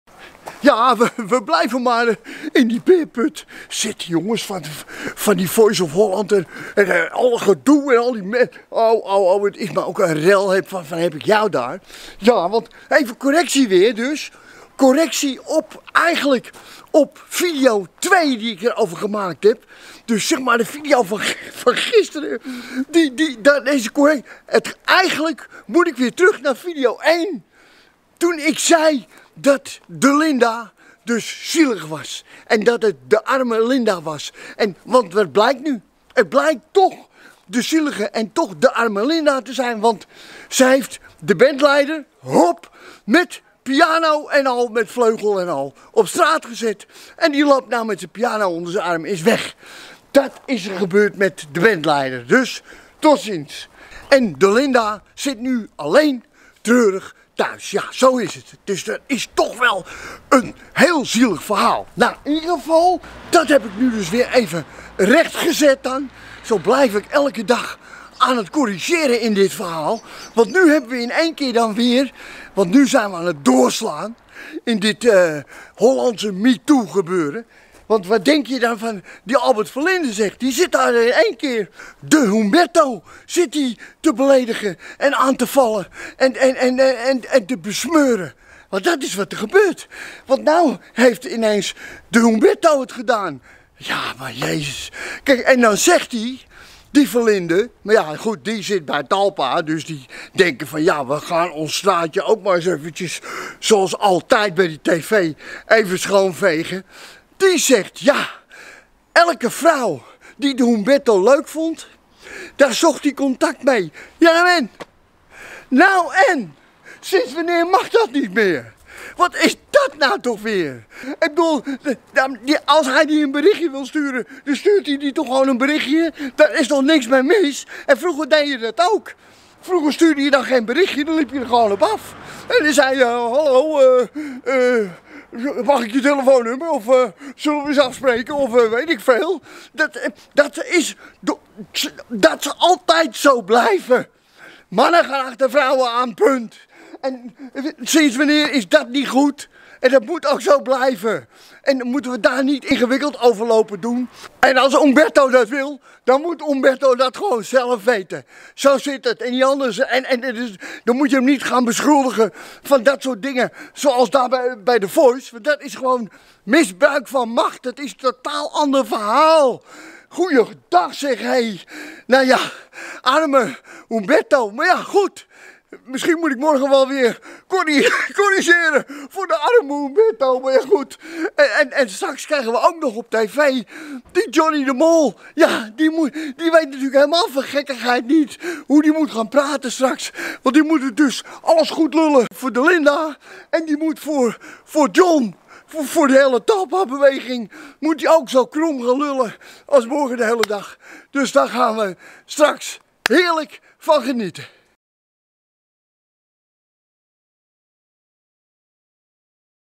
Arme, arme Humberto!
Nu blijkt De Linda toch de arme Linda te zijn. En opeens is De Humberto ook in beeld in het Hollandse Me Too gebeuren! We komen op het punt dat iedereen een beetje gaat doorslaan.